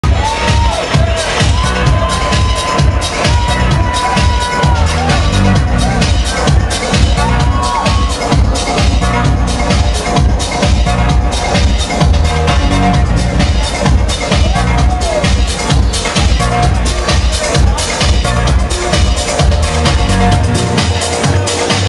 Sultan, Maher Daniel & Sean Keating - Halifax, NS
Sultan, Maher Daniel & Sean Keating playing Reflections Cabaret, Halifax NS